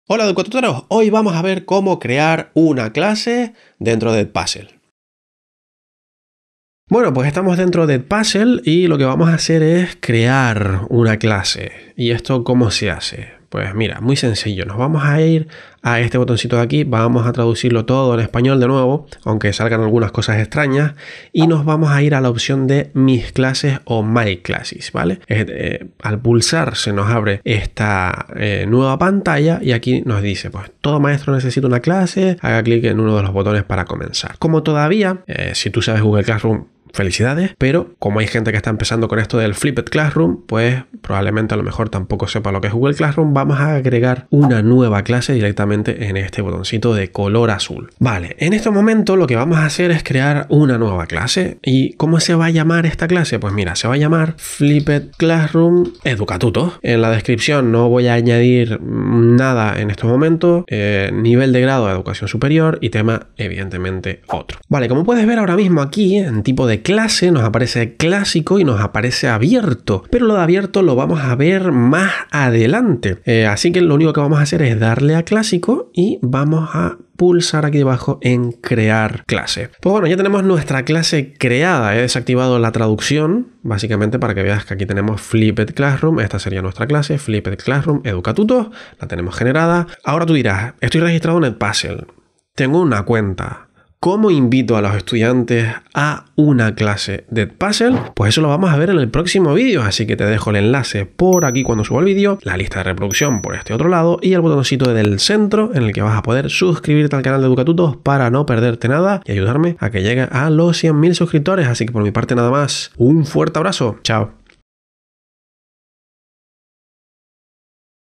🛠️Cómo CREAR una CLASE en EdPuzzle【2】Curso de EdPuzzle / Edpuzzle tutorial español
Aprende cómo crear tu clase dentro de EdPuzzle paso a paso.

⚫ Mis recomendados de AMAZON
Compra desde este enlace y me ayudarás a seguir creando contenido